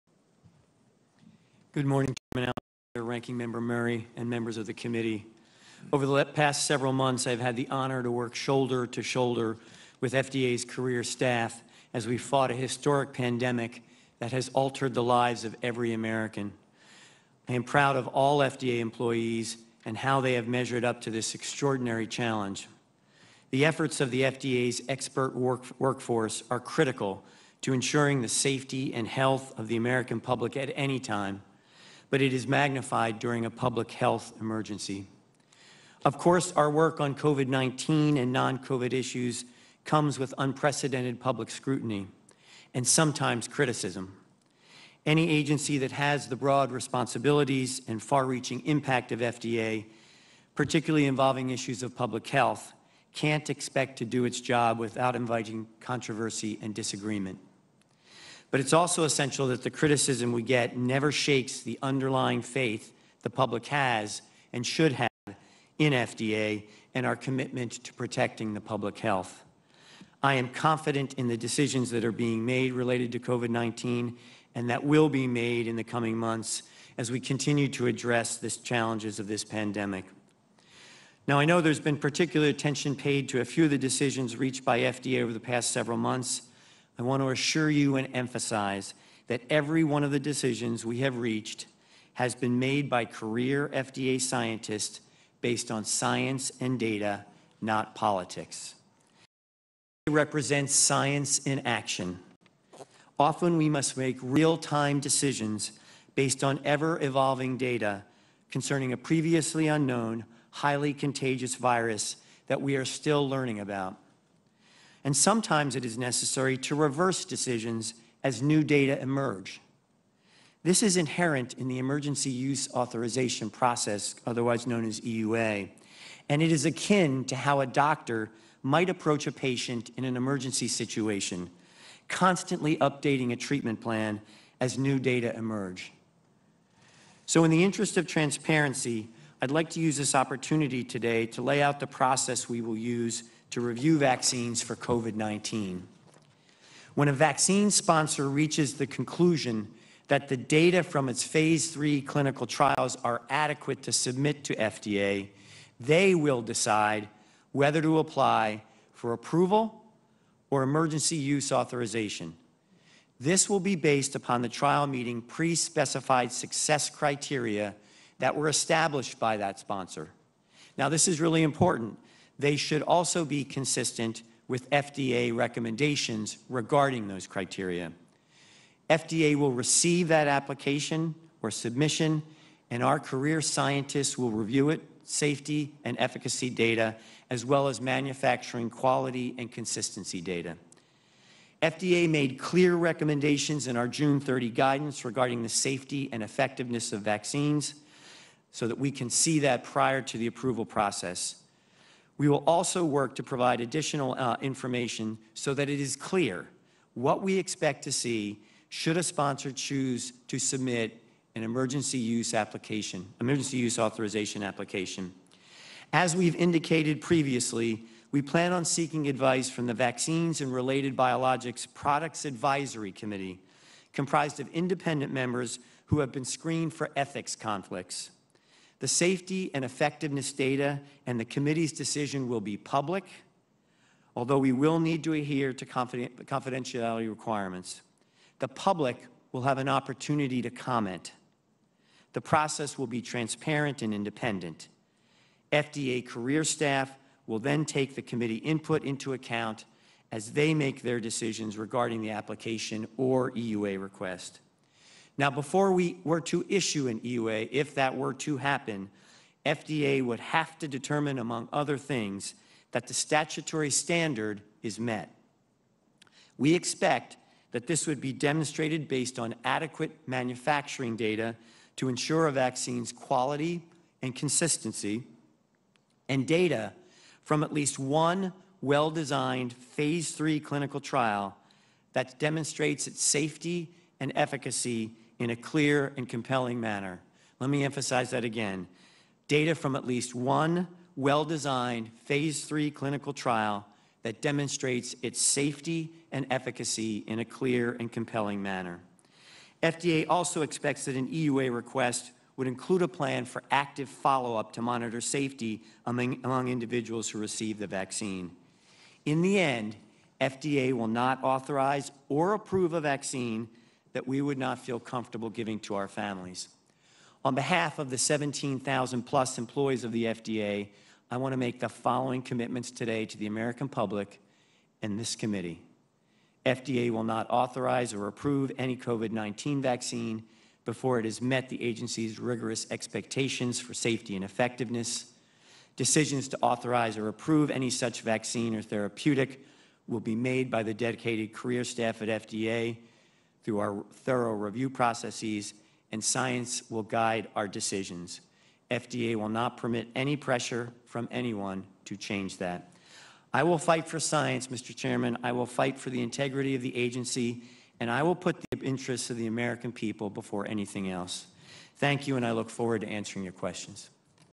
FDA director won’t approve vaccine "would not feel comfortable giving to our families" (2020)
Food and Drug Administration (FDA) Director Stephen Hahn defended his organization’s actions during the novel coronavirus pandemic during his opening remarks to the Senate Committee on Health, Education, Labor and Pensions (HELP) Wednesday.

Hahn said the FDA would not authorize or approve any vaccine that “we would not feel comfortable giving to our families,” adding that any vaccine that is approved will have to meet rigorous expectations for safety and effectiveness.

Hahn also said all decisions regarding vaccine approval would be made by FDA officials, and that it "would not permit any pressure from anyone to change that." Global News September 23, 2020

October, 2020: US FDA issues an emergency use authorization for COVID-19 vaccines

October 22, 2020: The Data and Safety Monitoring Boards (DSMBs) will vote on which vaccines get approved contain members from pharmaceutical companies making vaccines waiting on approval. The board also includes Dr. Anthony Fauci

October 22, 2020: FDA Safety Surveillance of COVID-19 Vaccines; DRAFT Working list of possible adverse event outcomes (including disease and death)

November 10, 2020: A Phase 1/2/3 Study to Evaluate the Safety, Tolerability, Immunogenicity, and Efficacy of RNA Vaccine Candidates Against COVID-19 in Healthy Individuals

December 10, 2020: Vaccines and Related Biological Products Advisory Committee Meeting